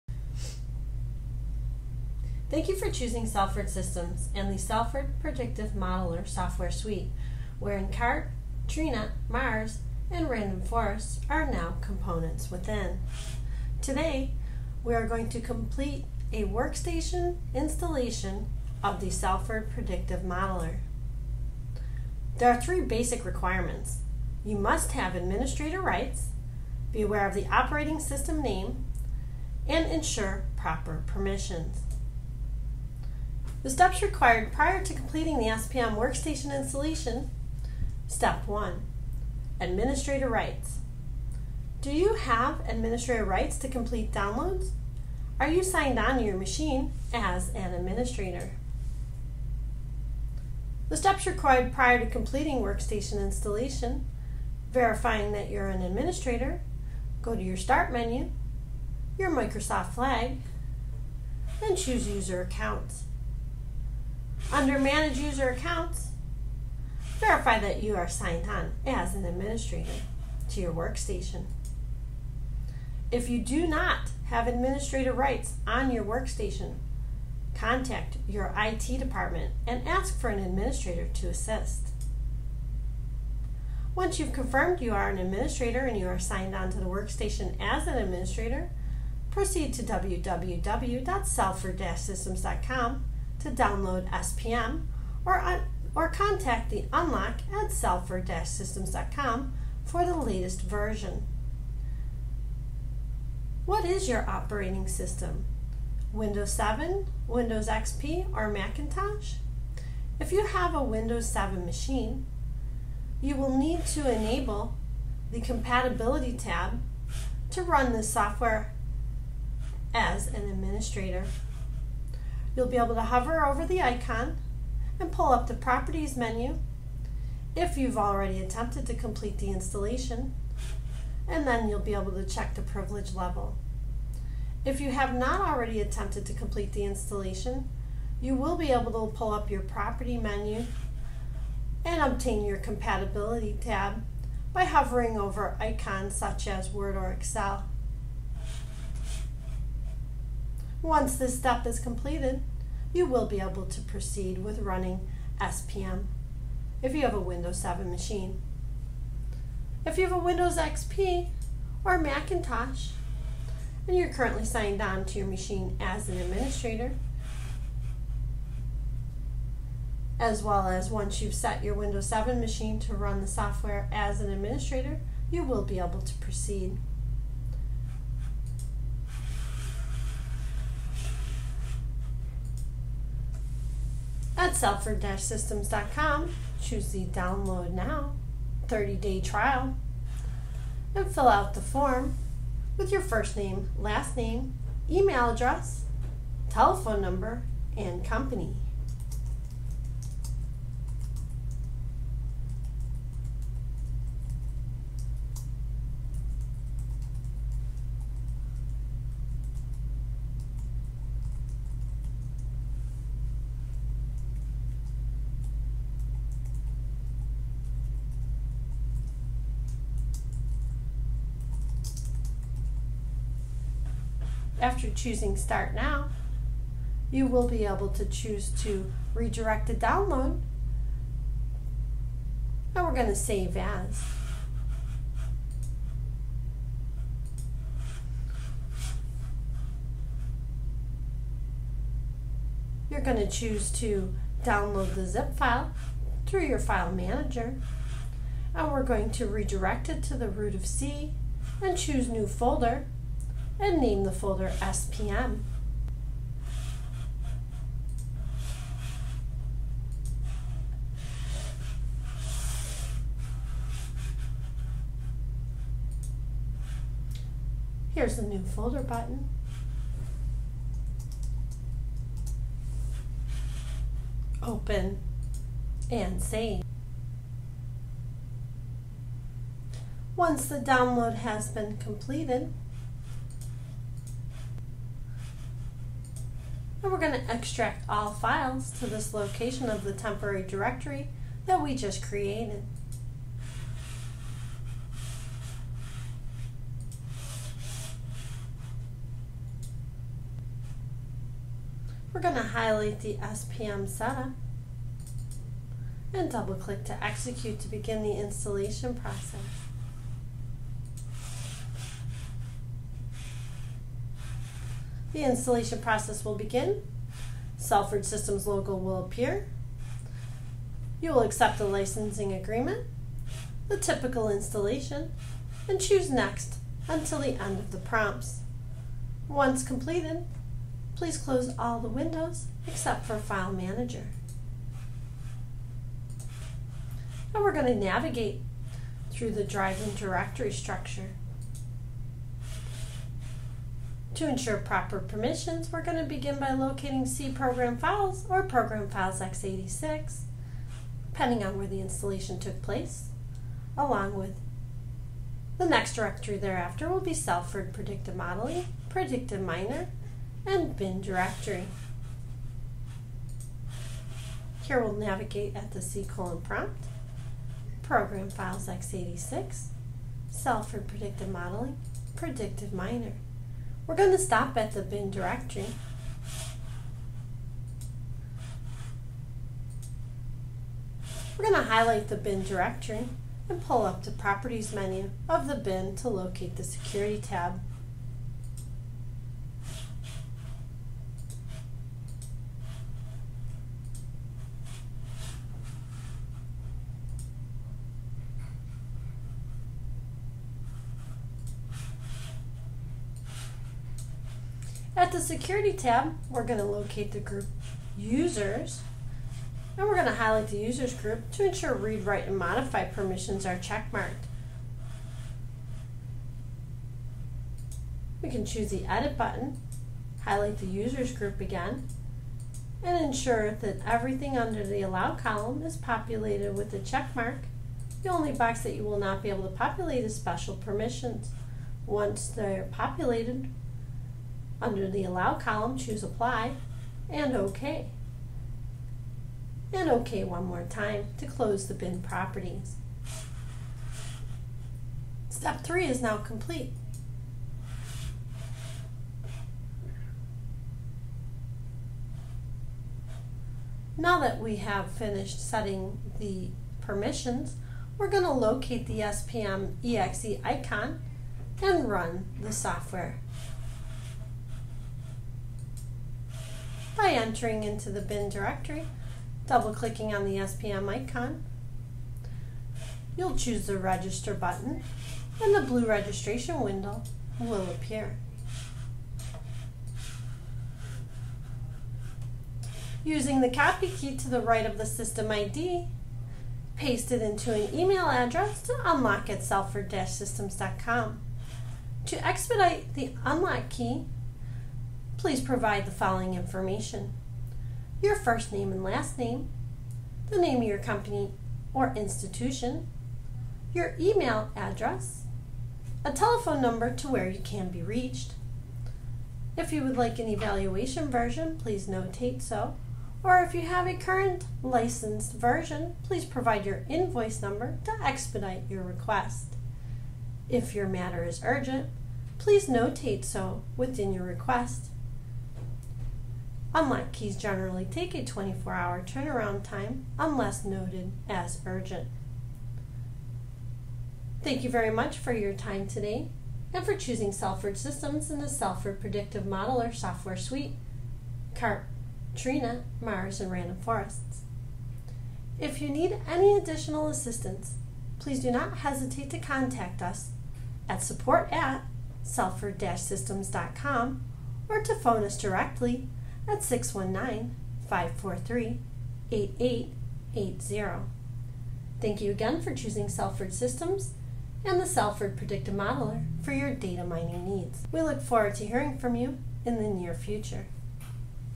How to Install The Salford Predictive Modeler on a Workstation
This tutorial will help you install the SPM software suite on your workstation.